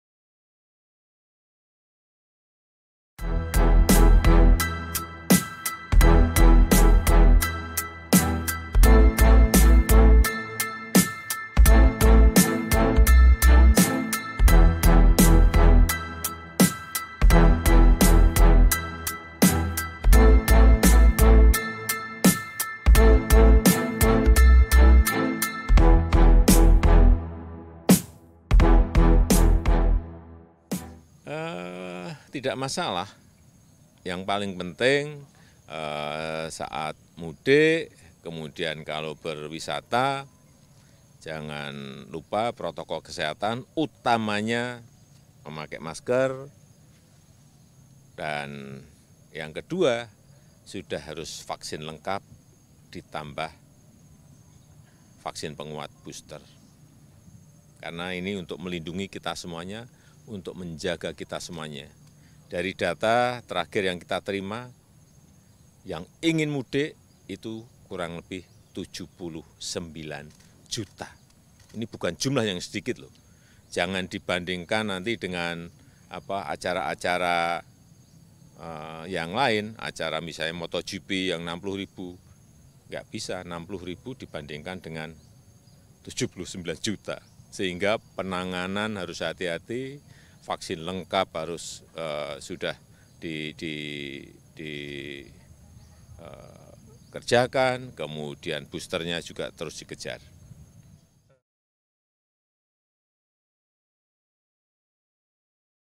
Jokowi Buka Suara Soal Heboh Syarat Mudik Beda dengan Nonton MotoGP
JAKARTA, KOMPAS.TV - Presiden Jokowi mempersilakan bagi masyarakat untuk mudik dan berwisata di lebaran tahun 2022 ini, namun dengan syarat. Syarat mudik tahun ini yaitu dengan menerapkan protokol kesehatan dan vaksin lengkap serta booster.

"Tidak masalah yang paling penting saat mudik kalau berwisata jangan lupa protokol kesehatan utamanya memakai masker dan yang kedua sudah harus vaksin lengkap ditambah booster karena ini untuk melindungi kita semua dan menjaga kita semua," ujar Presiden Jokowi usai meninjau fasilitas pariwisata Candi Borobudur pada Rabu, 30 Maret 2022.

Baca Juga Presiden Jokowi Minta Masyarakat yang Berkunjung ke Tempat Wisata saat Mudik Tetap Disiplin Prokes di

Presiden Jokowi juga meminta tidak dibandingkan dengan syarat acara lain seperti MotoGP.

"Yang ingin mudik kurang lebih 79 juta jangan dibandingkan dengan acara cara yang lain misalnya motogp yang 60 ribu dibandingkan dengan 79 juta," kata Jokowi.